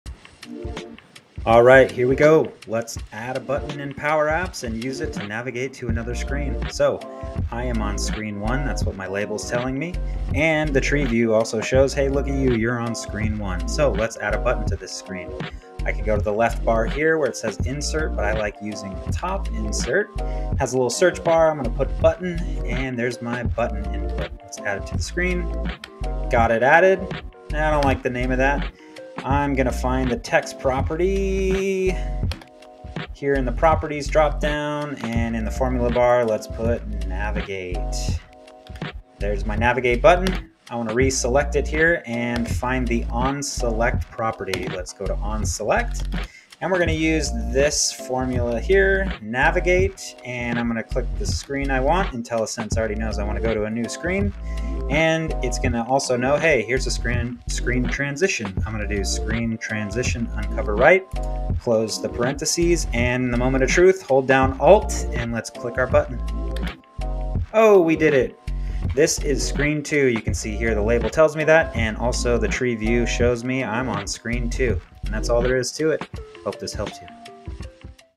Power Apps: Add a Button & Navigate to a New Screen in 60 Seconds ✅
Need to add a button in Power Apps and make it navigate to another screen? In this quick 1-minute tutorial, I’ll show you how to use the Navigate function to switch screens in a Canvas App.